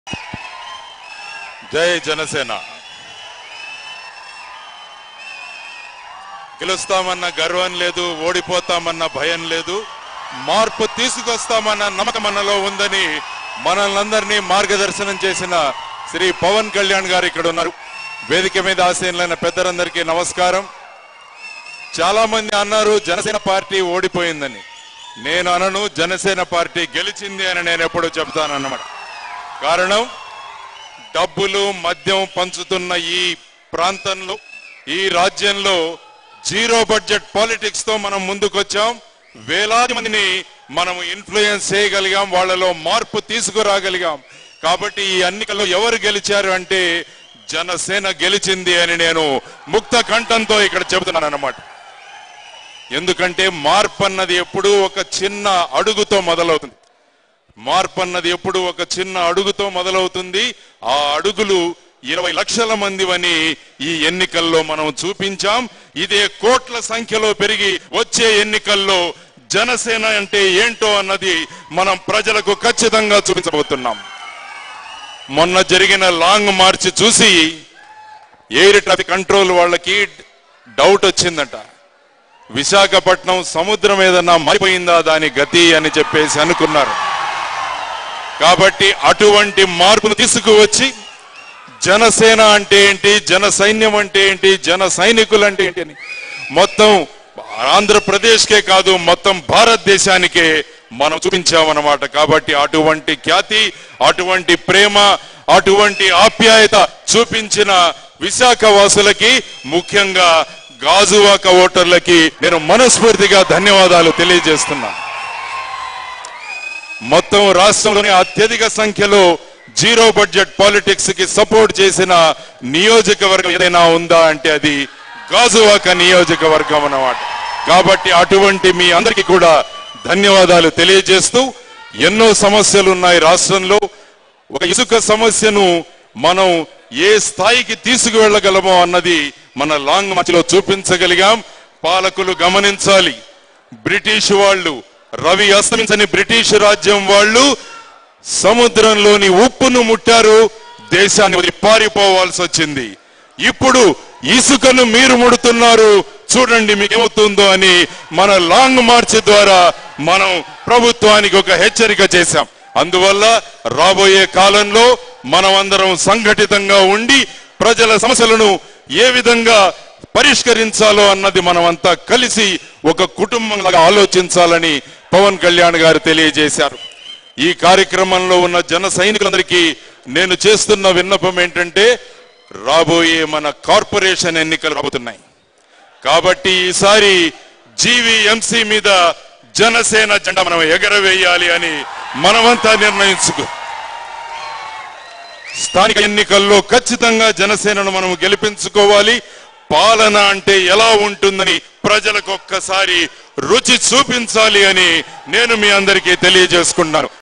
JD Lakshmi Narayana Strong Counter To Ys Jagan | Pawan Kalyan Meeting with Gajuwaka Leaders | CC
Watch Here JD Lakshmi Narayana Strong Counter To Ys Jagan Video Here